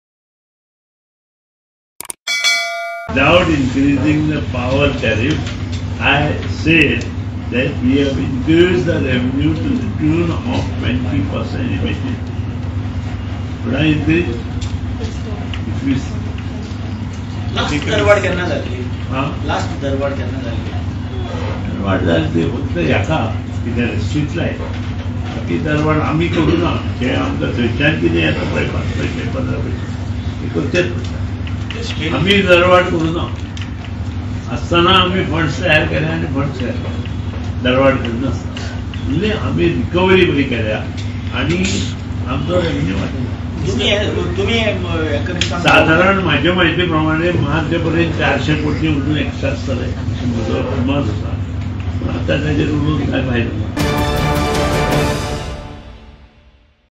Power department increased revenue by 20 % through recovery: Power Min
Power Minister Sudin Dhavlikar on Wednesday claimed that the power department has increased its revenue to the tune of 20% through recovery, without increasing the tariff. Minister further said that by March 2024 extra revenue of Rs 400 crores is likely to be added to the state treasury. When queried over the Tamnar power line project, he said that the project is on and as and when that happens, Goa and other states will benefit from it.